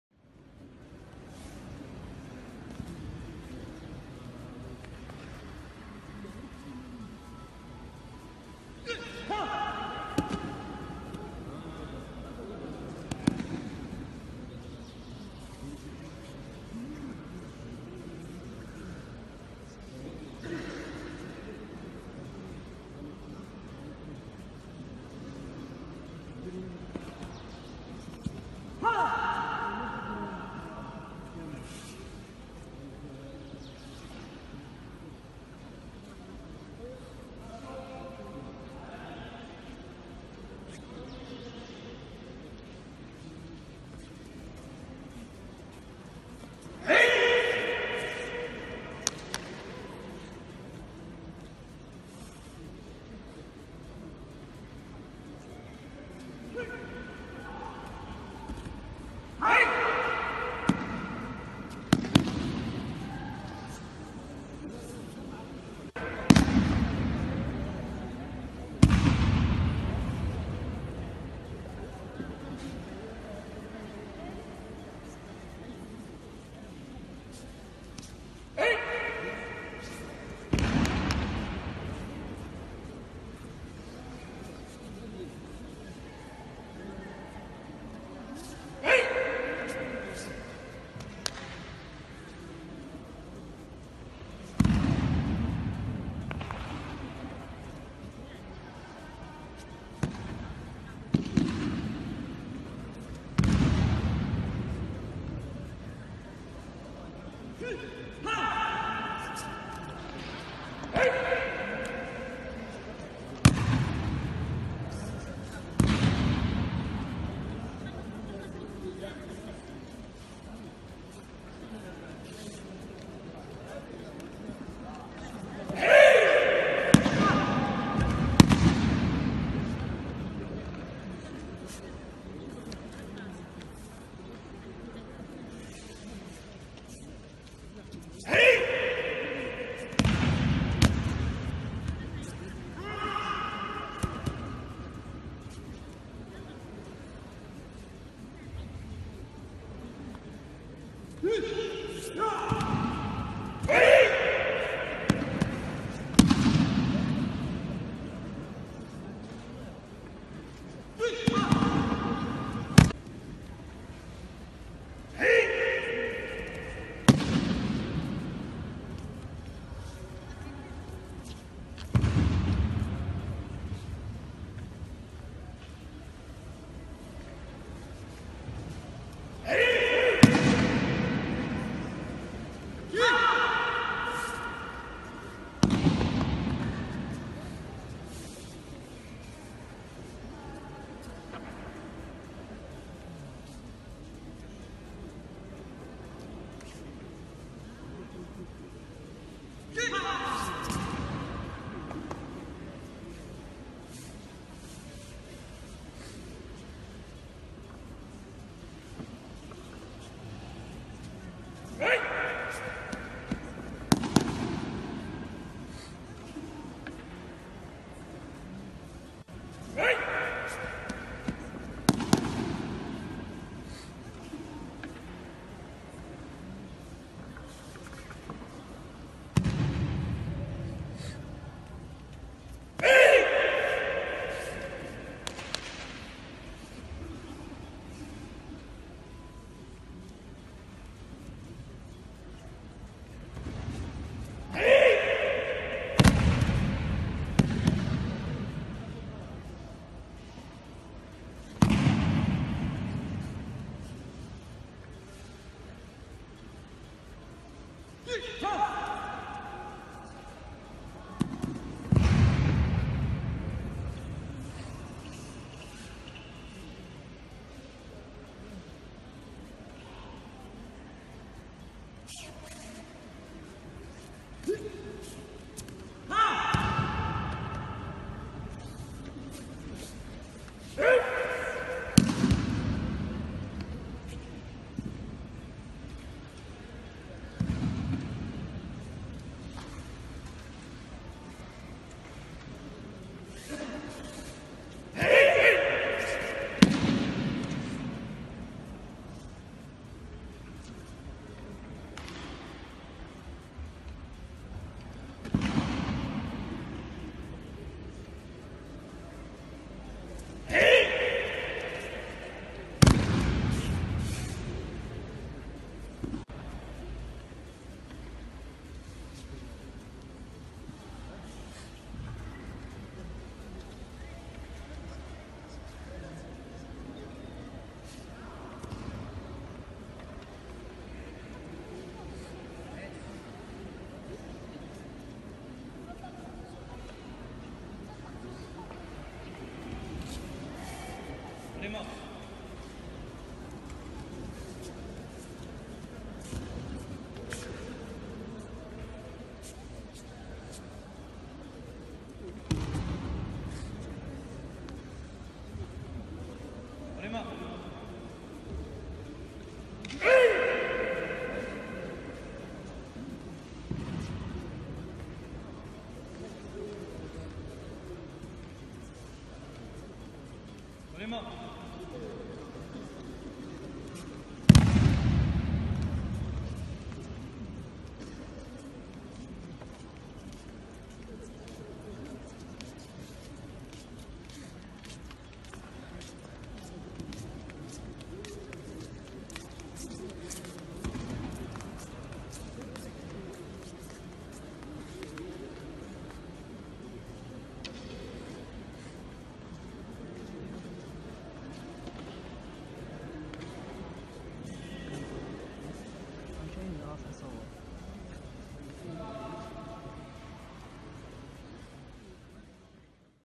Championnat Du Maroc Goshinjitsu No Kata
CLUB BUDOKAN DU MAROC dans le championnat du maroc de judo 2ème place